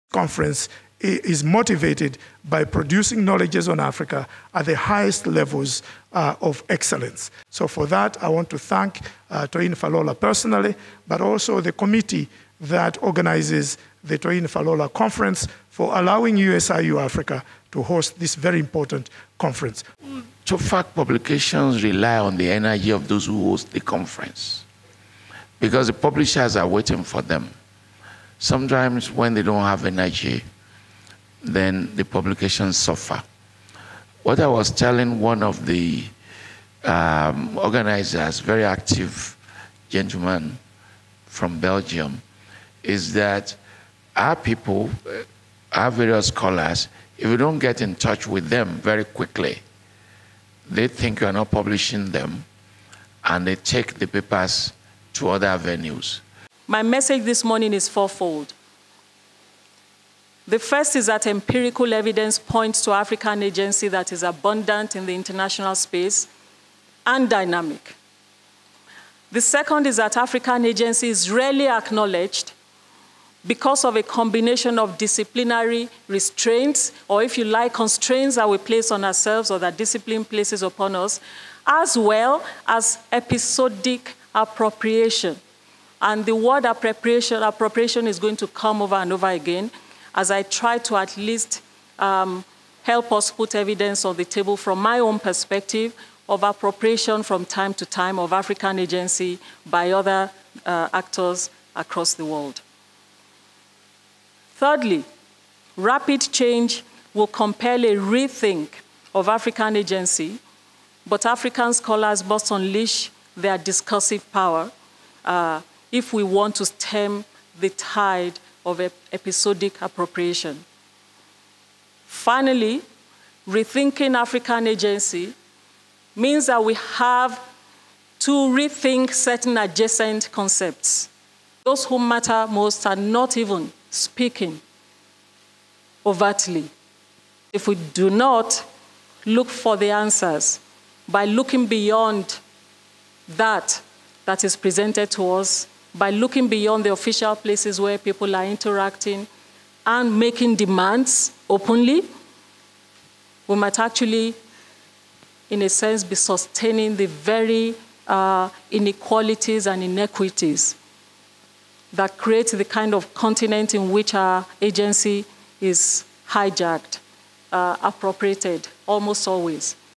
Eigth Toyin Falola Annual International Conference on Africa and the African DIaspora (TOFAC) 2018
Opening remarks of The Eighth Toyin Falola Annual International Conference on Africa and the African Diaspora - TOFAC - 2018, held at the USIU-Africa Auditorium.

The conference seeks to capture Africa’s process of innovating the future for itself based on a progressive embrace of what Africa is:  young, productive, optimistic, and endowed with a state of transformation that is not only confident about itself, but also unapologetic about its global outlook.